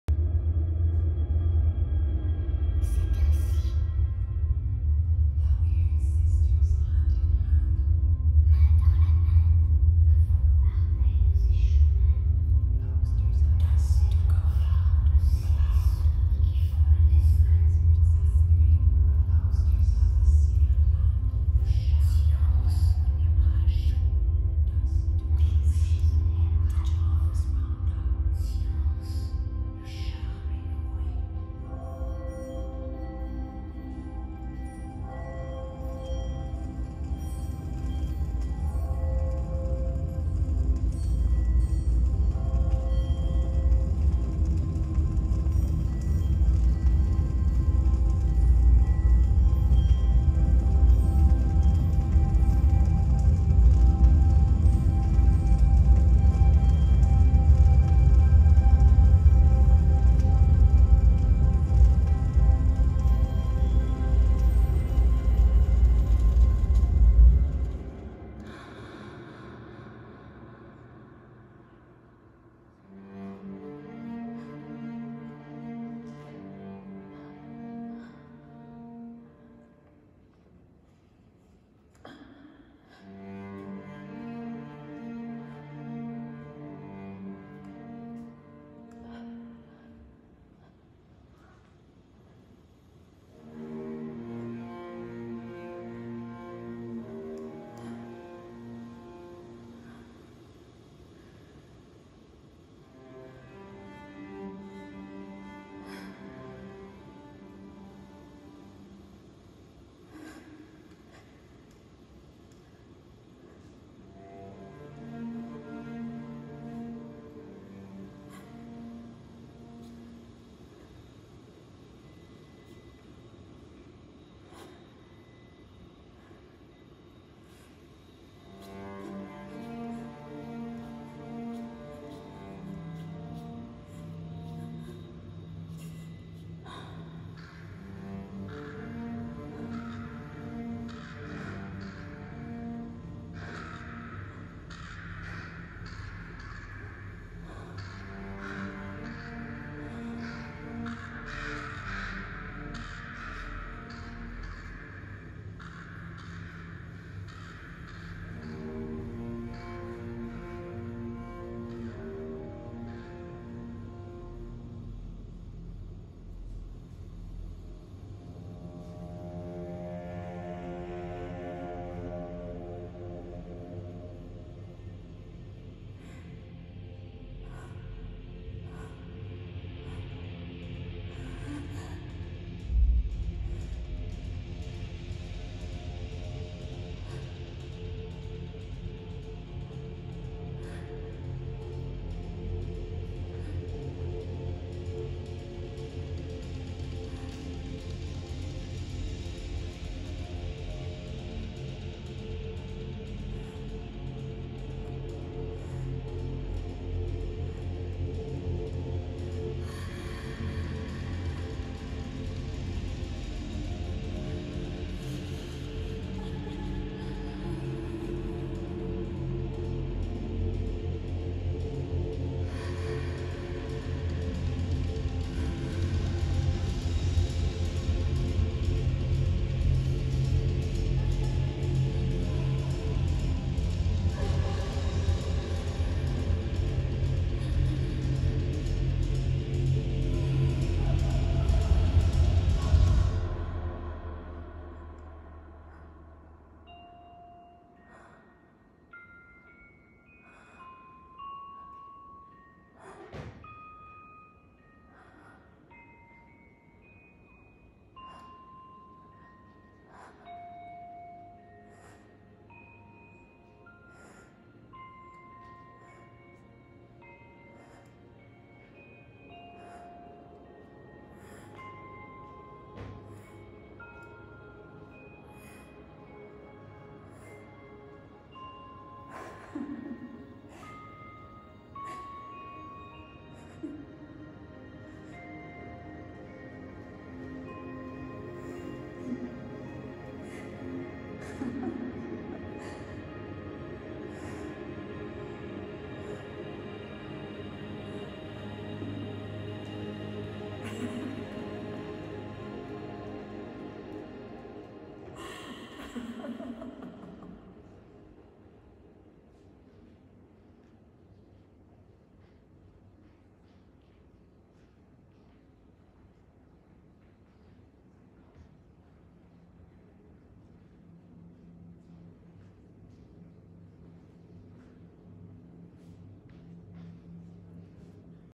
MICHEL LEMIEUX The Disappearance of Lady Macbeth
hologram installation , 6:30 min

THIERRY MUGLER exxhibition Munich Kunsthalle 2020